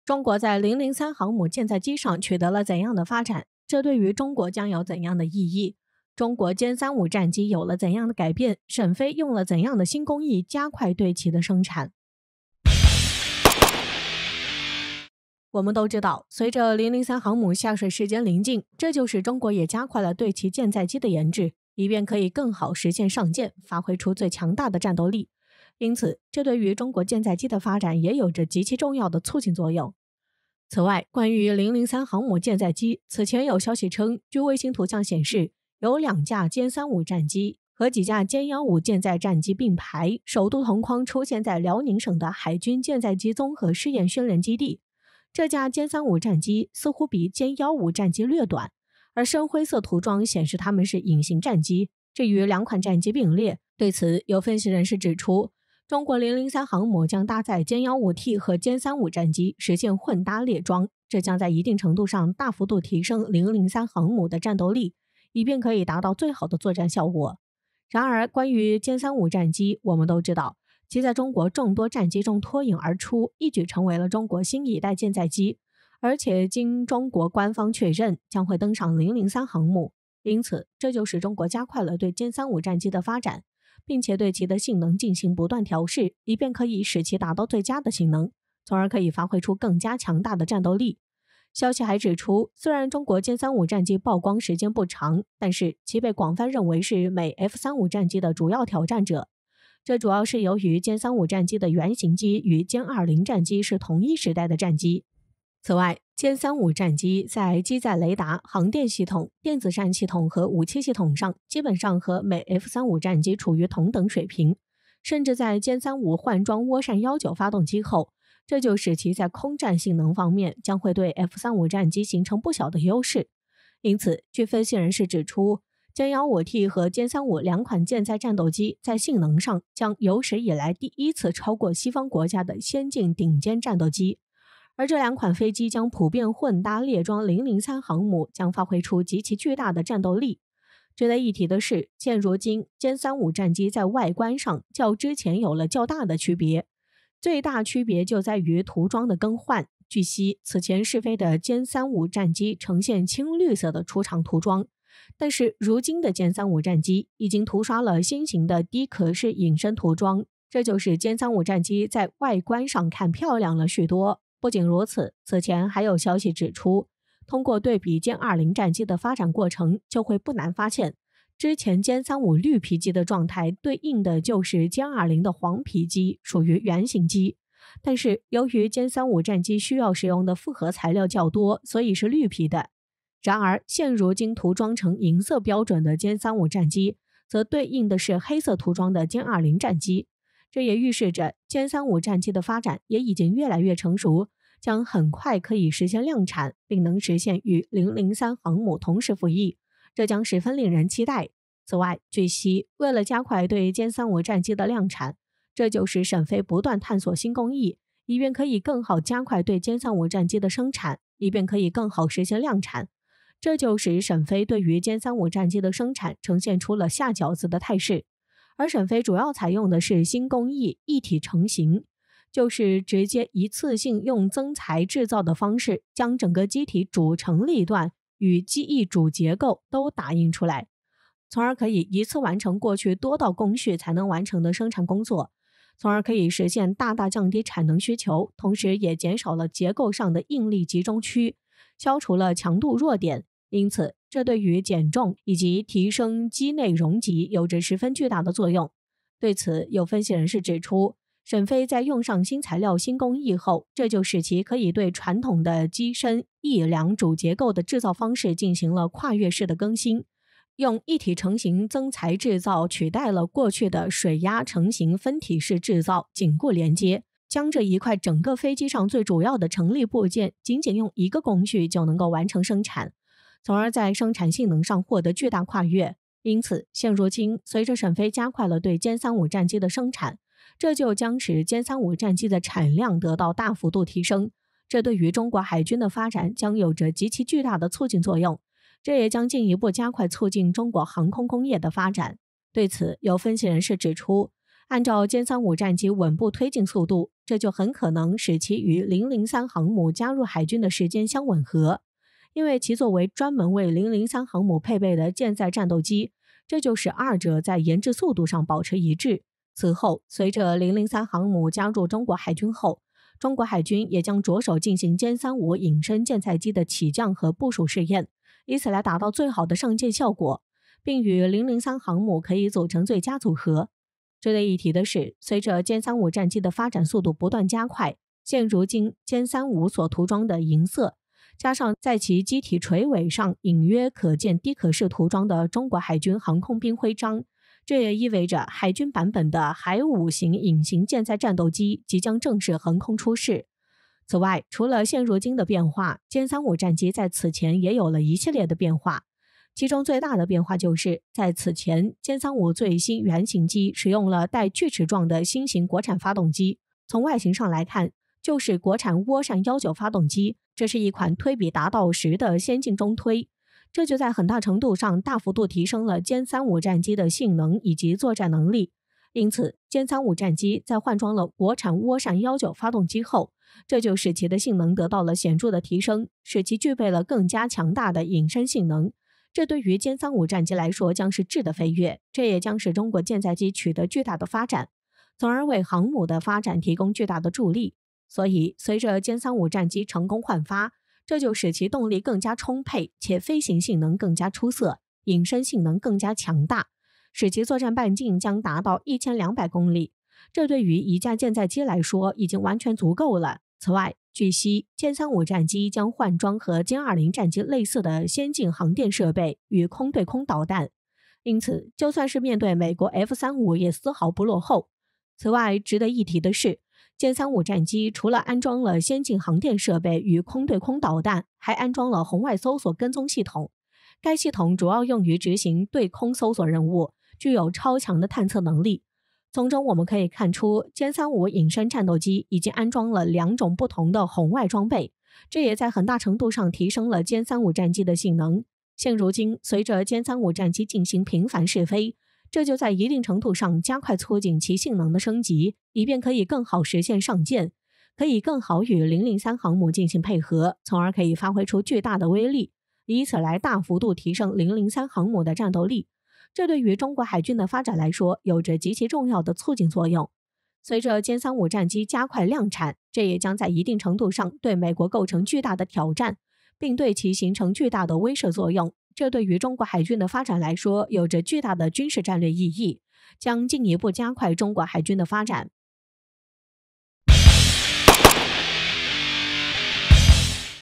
震撼！ 003航母即將下水之際，殲-35再亮相，沈飛用上新工藝，加快量產，形成“下餃子”態勢，將大幅提升航母戰力！【一號哨所】
中國在003航母艦載機上，取得了怎樣的發展，這對於中國將有怎樣的意義？中國殲-35戰機，有了怎樣的改變，沈飛用了怎樣的新工藝加快對其的生產？
震撼！ 003航母即將下水之際，殲-35再亮相，沈飛用上新工藝，加快量產，形成“下餃子”態勢，將大幅提升航母戰力！
一號哨所 殲35 003航母 我們的影片會遵循公平、尊重的原則，確保不會包含任何仇恨言論的元素。